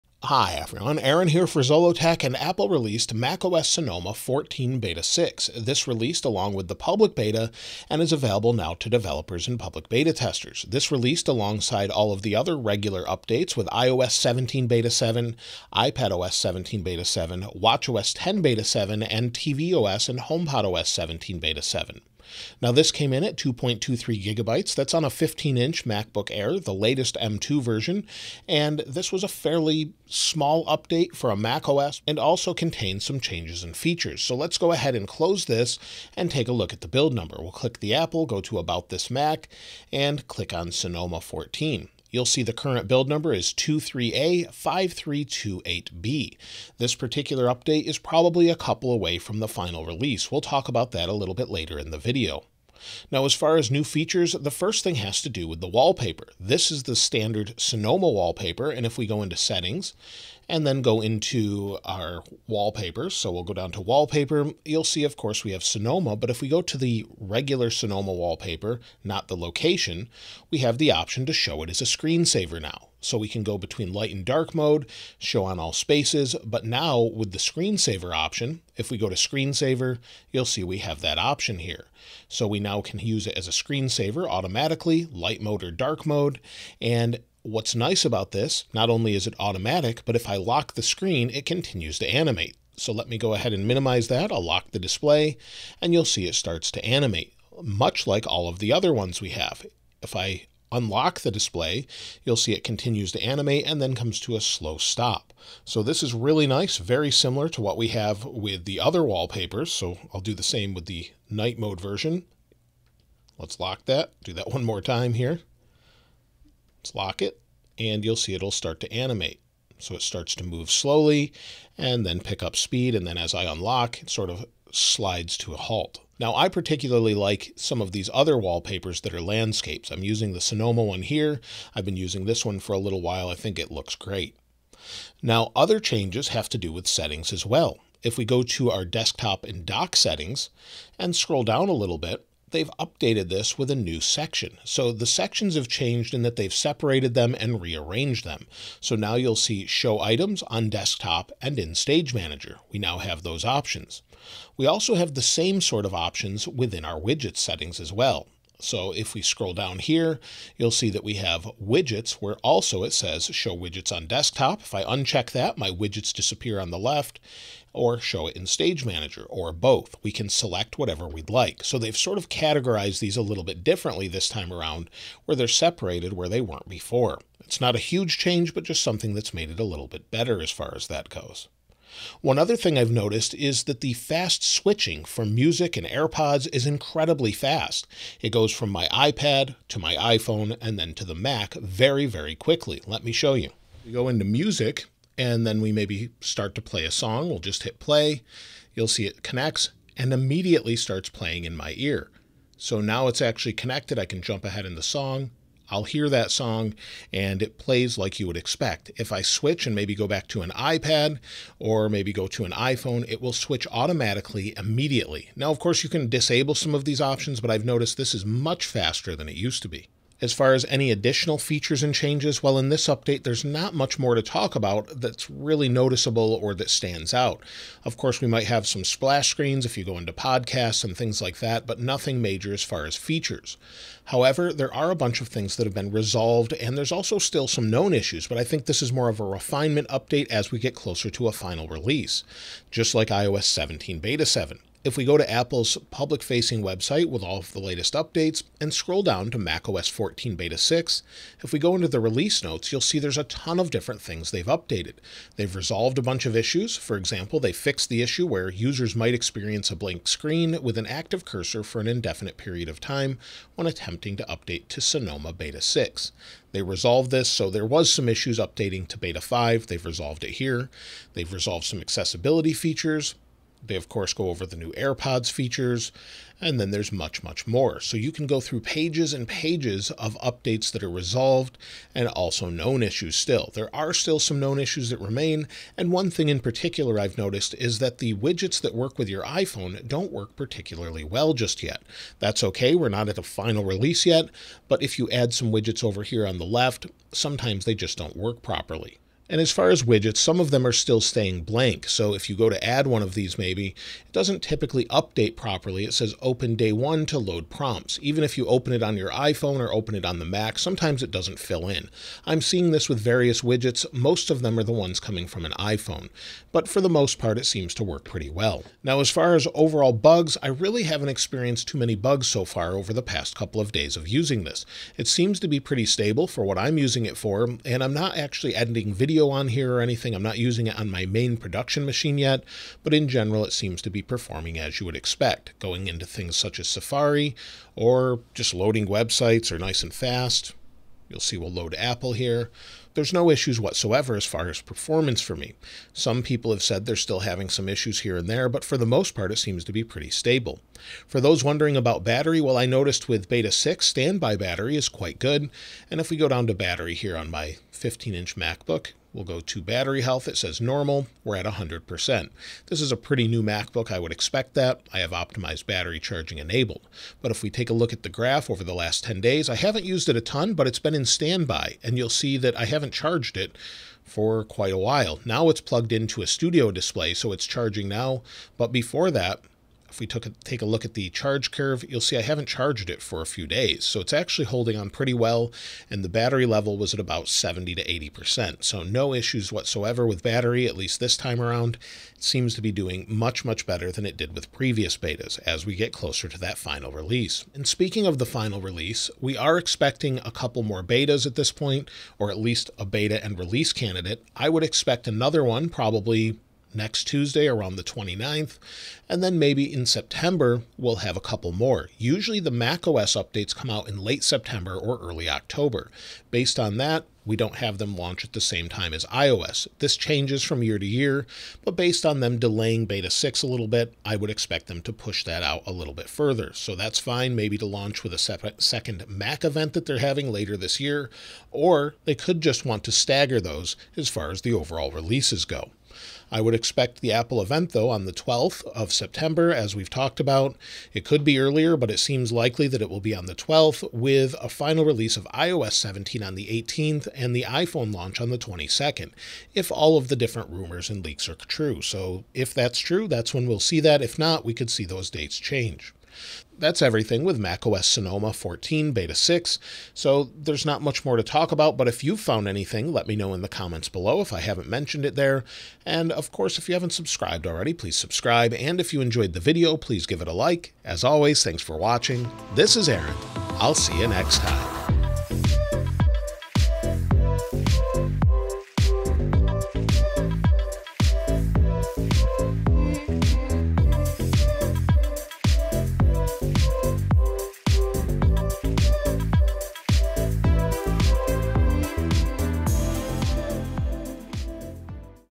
macOS 14 Sonoma Beta 6 is Out! - What's New?
macOS 14 Sonoma Beta 6 Released and MacOS Sonoma 14 Public Beta 4 is out. The 6th macOS 14 Beta update adds updates and changes with Wallpaper and Settings. In this video I show you everything new with macOS 14 Sonoma Beta 6 using the new 15 inch MacBook Air.

Anker is The Official and Exclusive Charging Partner of my channel.

*Chapters*
00:00 - Everything New
00:05 - Apple Also Released
00:22 - Size
00:39 - Build Number
00:52 - New Features, Changes and Updates
04:02 - Release Notes and Resolved Issues
04:50 - Bugs
05:32 - Performance
05:52 - Battery Life
06:37 - macOS Sonoma Final Release
07:35 - Conclusion